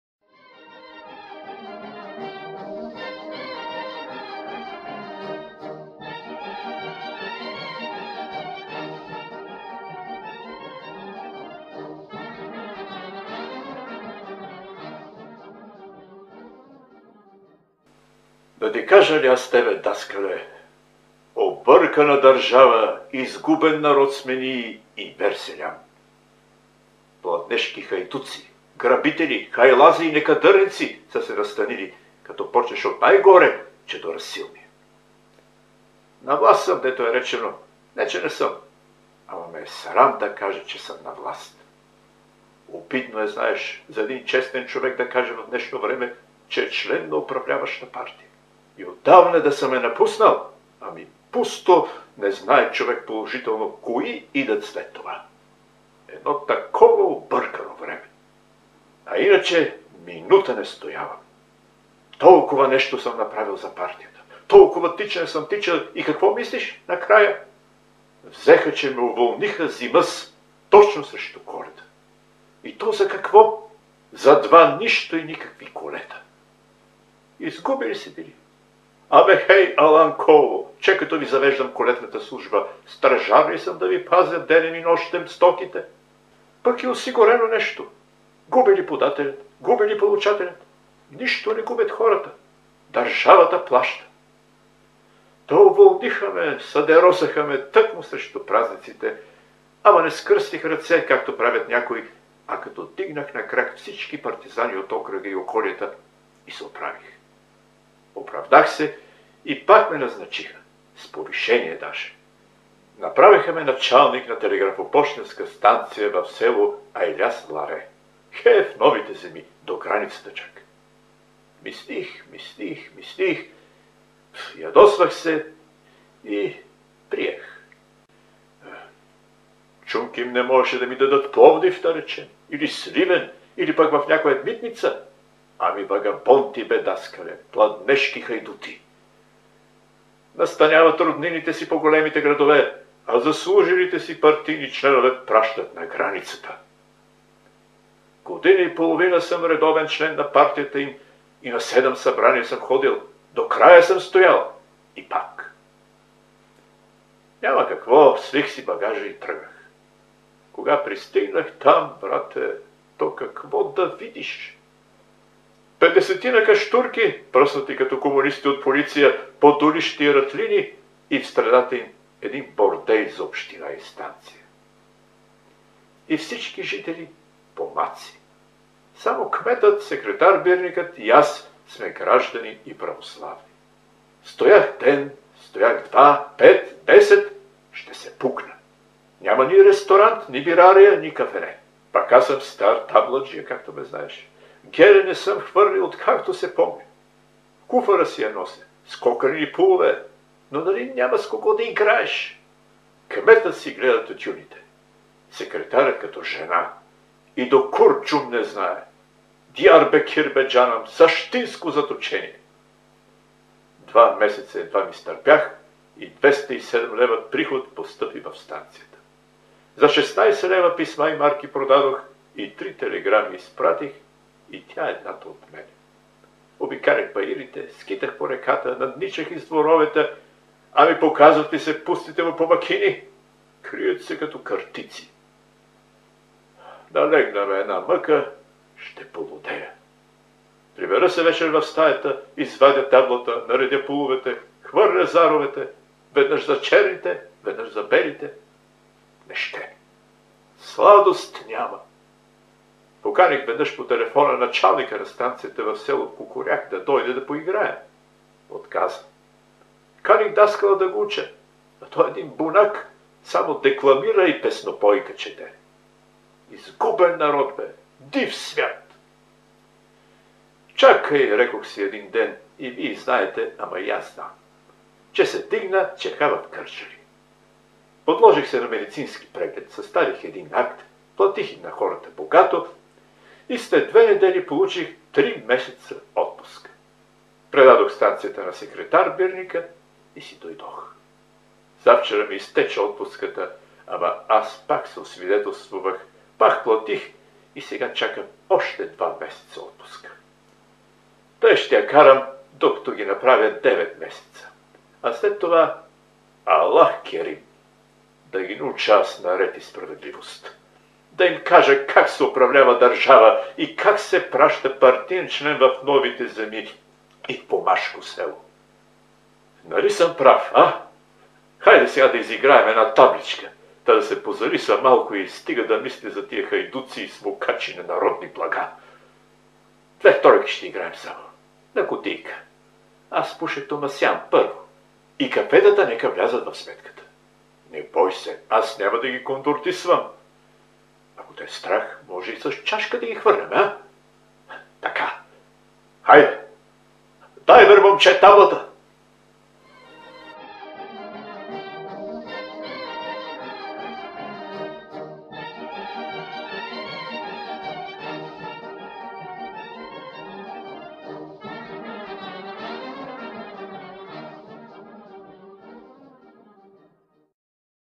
Чудомир - "Объркана държава", изп. Богдан Дуков
Димитър Христов Чорбаджийски (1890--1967) с псевдоним Чудомир е български писател-хуморист, краевед и художник.Първите му изяви в печата датират от 1907 г., когато негови карикатури и римувани злободневки биват публикувани във вестник Балканска трибуна; после следват първите му фейлетони, разкази, епиграми, публикувани от списанията Барабан, Остен, Жило, Червен смях, както и от вестниците Слово, Зора, Искра (Казанлък) и др. Чудомир придобива голяма популярност чрез кратките си хумористични разкази — те не слизат от читалищните сцени, а книгите му за кратко време претърпяват многобройни издания./ Chudomir (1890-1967) ist Bulgarische Schriftsteller, bekant mit seine Satirische Erzählungen und Feiletonen. "Verwirrter Staat"  - Darsteller und Inszenierung - Bogdan Dukov, 2019).